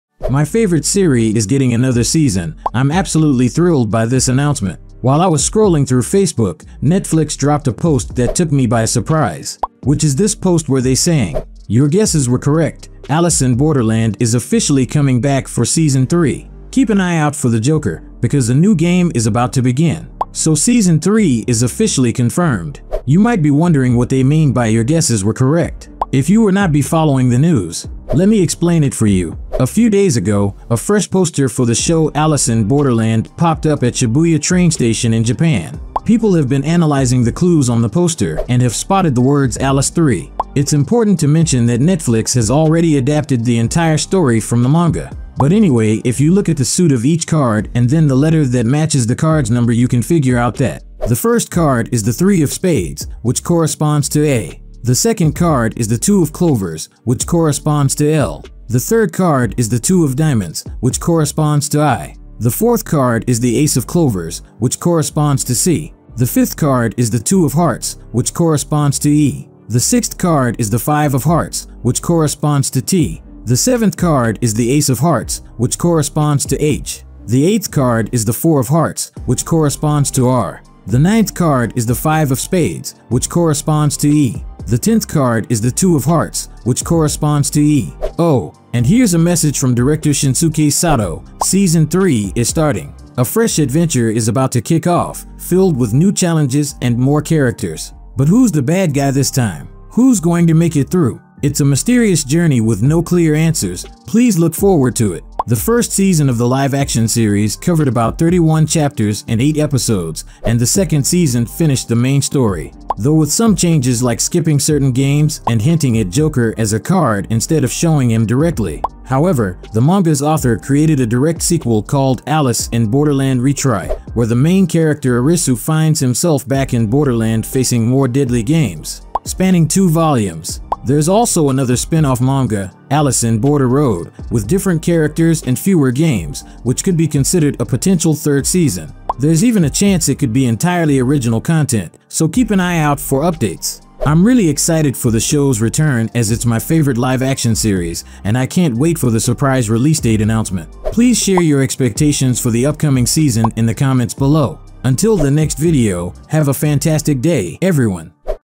Alice In Borderland Season 3 is OFFICIALLY CONFIRMED! By (Director & Netflix)
In this video, we've got one exciting announcement to share: Alice In Borderland Season 3 is officially happening! Director and Netflix have given the green light, and we're here to break down what this means for fans of the series. Get ready for more twists, turns, and mind-bending challenges in the Borderland!